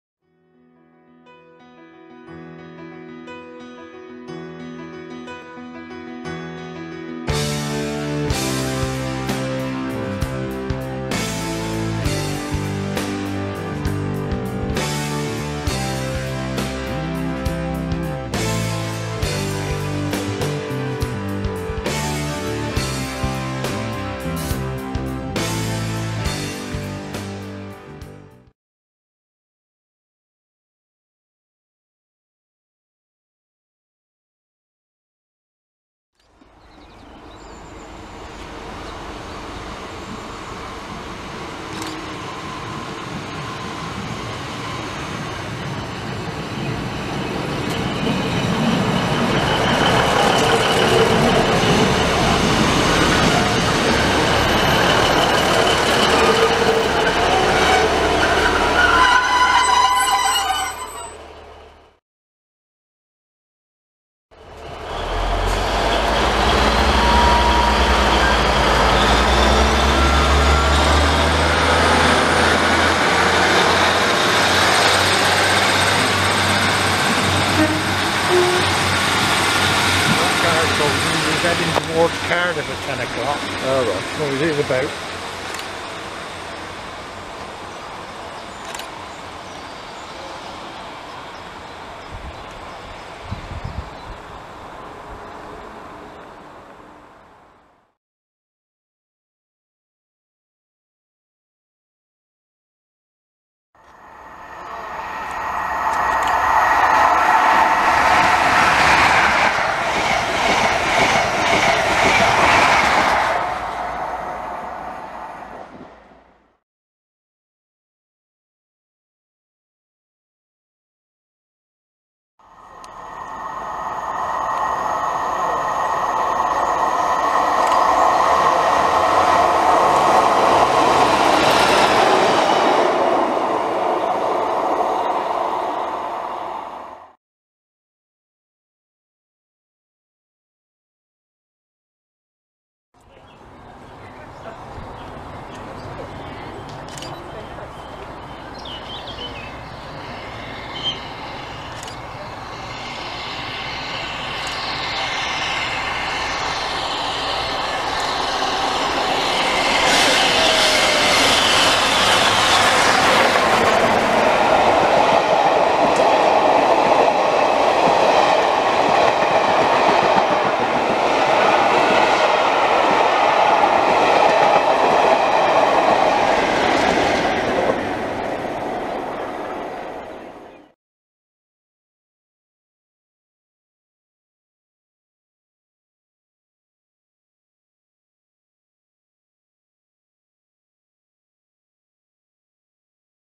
Waiting for 60103 Flying Scotsman @ Cam & Dursley 2017!
Finally seen her in steam! I heard Flying Scotsman was in the area so I headed down to a surprisingly quiet Cam & Dursley to see her on a run from Gloucester to Bristol Temple Meads with some sprinters & HSTs while I waited!